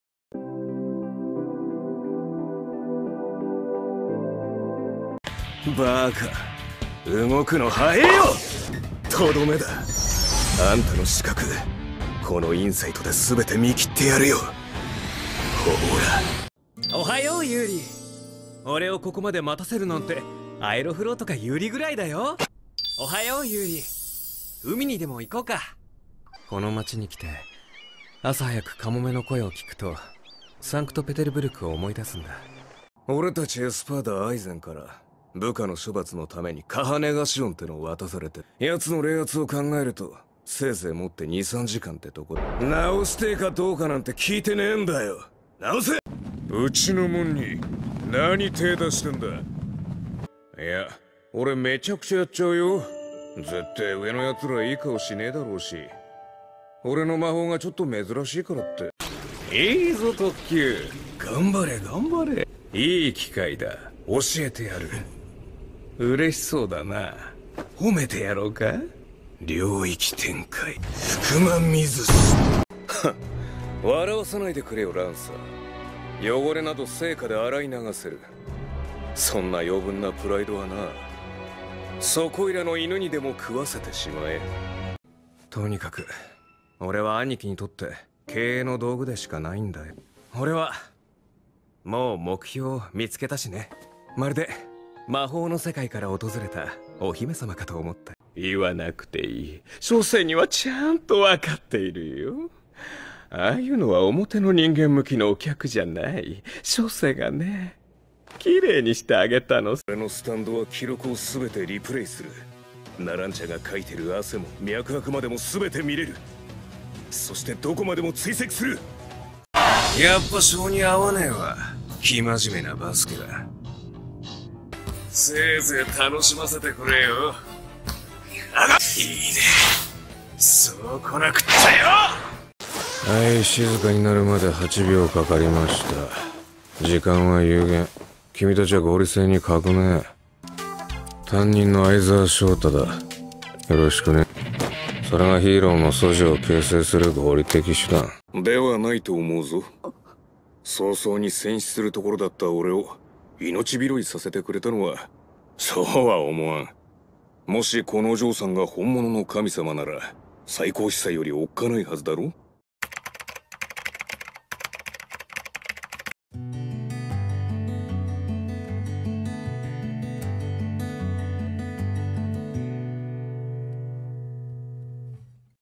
Yami Sukehiro same Japanese Voice Actor (Junichi Suwabe) as Ryomen Sukuna and Shōta Aizawa
Anime Characters voiced by Junichi Suwabe